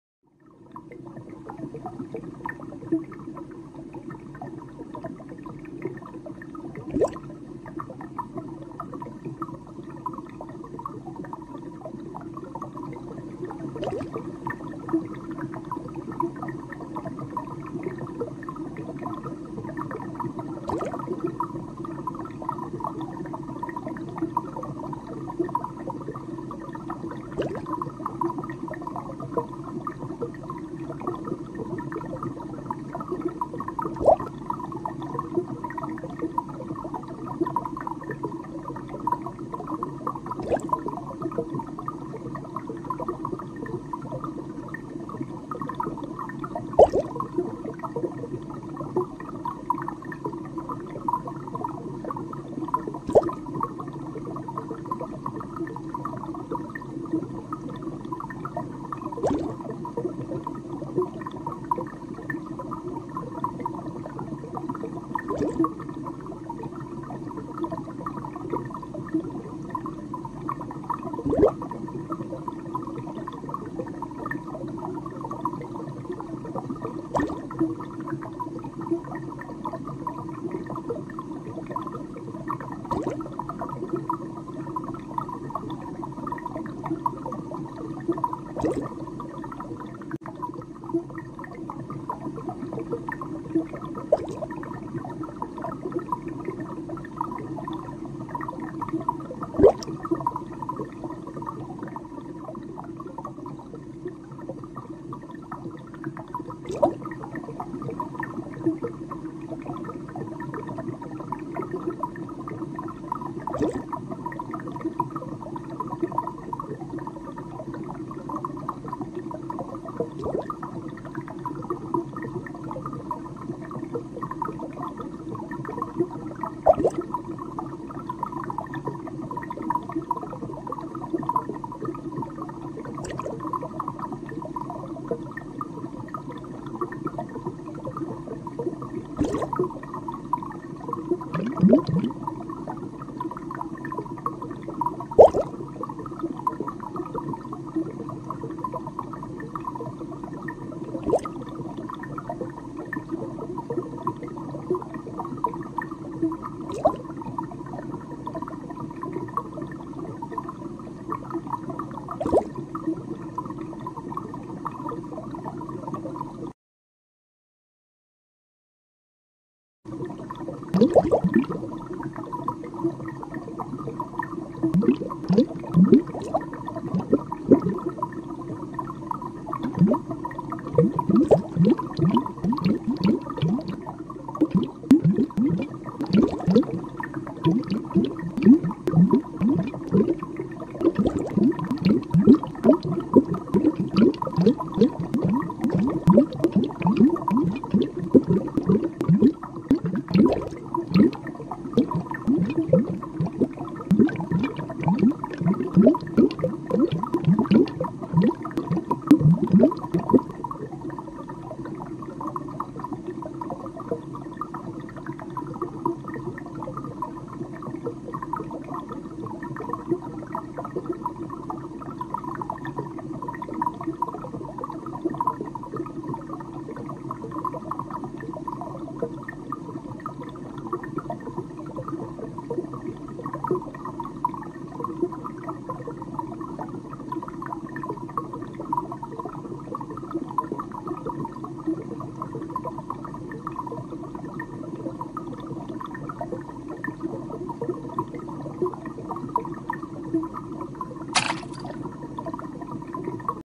Anime girl tied underwater
Anime girl tied underwater and drowns.